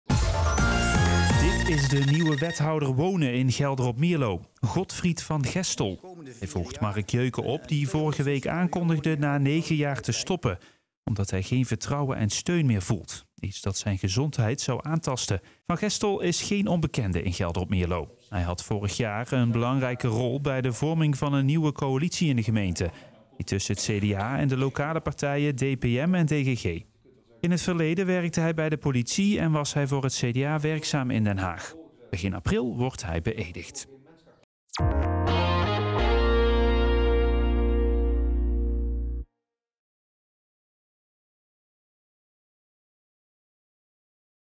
Godfried van Gestel kijkt uit naar nieuwe uitdaging in Geldrop-Mierlo: 'Ik voel me hier thuis'
Hij heeft nog even een paar dagen vakantie, maar Godfried van Gestel, de nieuwe wethouder bouwen en wonen in Geldrop-Mierlo, staat in de startblokken om te beginnen aan zijn nieuwe job. Als opvolger van Marc Jeucken, die binnenkort stopt, heeft hij er ontzettend veel zin in. "Het is hier zeker geen slangenkuil. Iedereen wil het beste voor Geldrop-Mierlo"